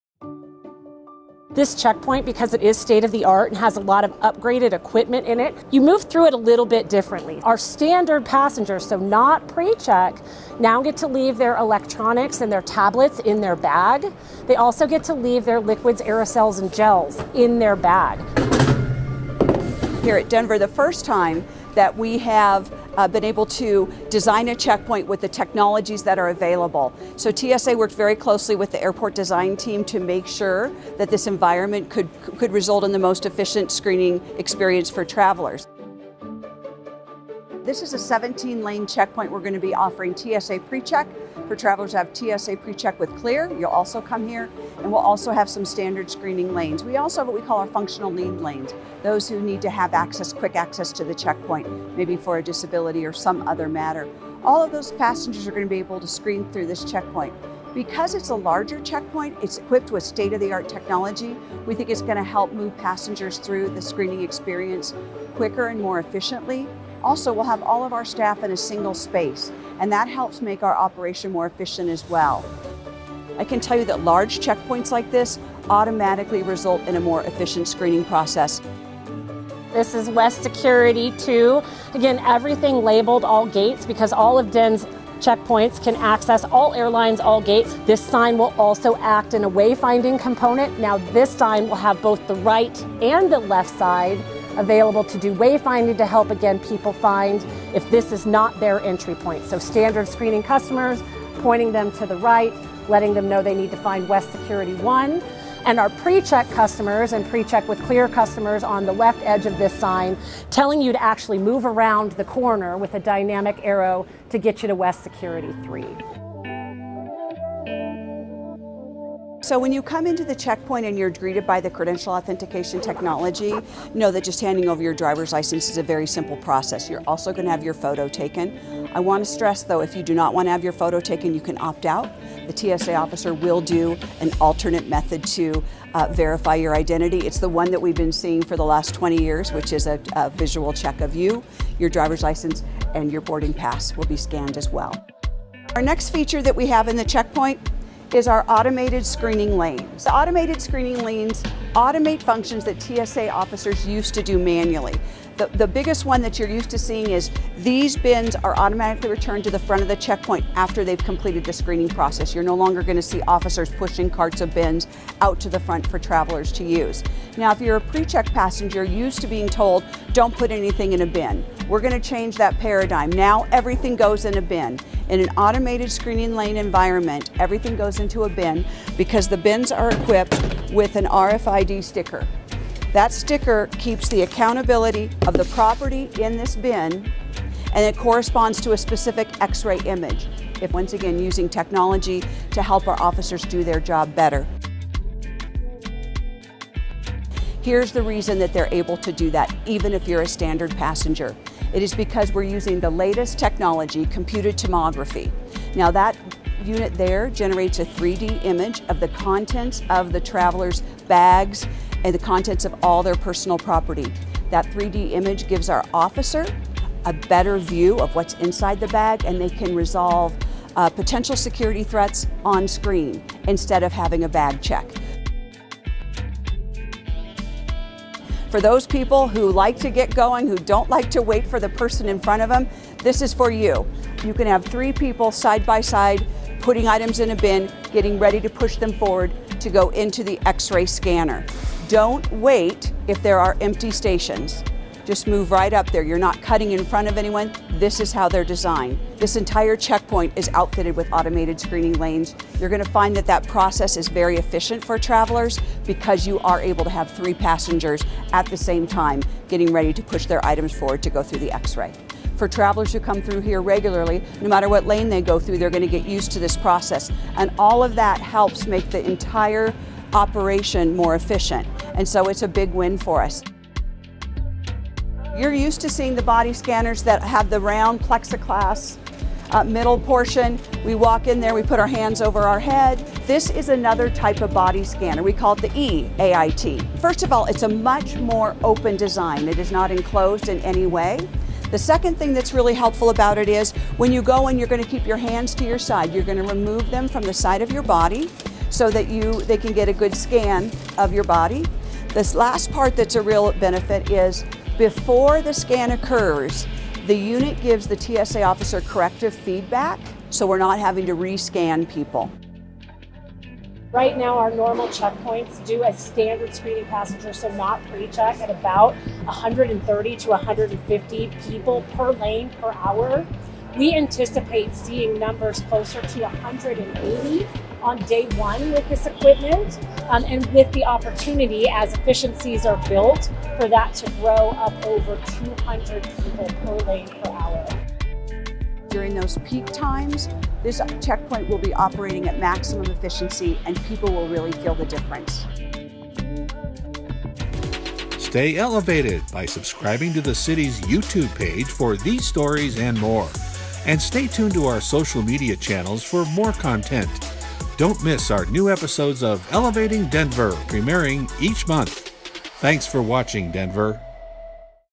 New Denver International Airport Security Checkpoint Opening Soon!
Excited about a smoother security screening process next time you fly through Denver? Learn about all the new features of the soon-to-be open West Security Checkpoint at Denver International Airport!

Automated screening lanes, dynamic signage, improved screening methods will all contribute to an additional 50 travelers per lane, per hour, passing through the checkpoint.

What new feature do you think will improve the experience the most?

00:00 - State of the art checkpoint
01:20 - Dynamic Signage to help with wayfinding
02:00 - Digital Identification
02:25 - Automated Screening Lanes
02:45 - Everything in a bin, nothing comes out of your bag
03:20 - 3D bag check image
03:50 - 3 loading areas per lane
04:35 - New body scanners
05:15 - More passengers per lane, per hour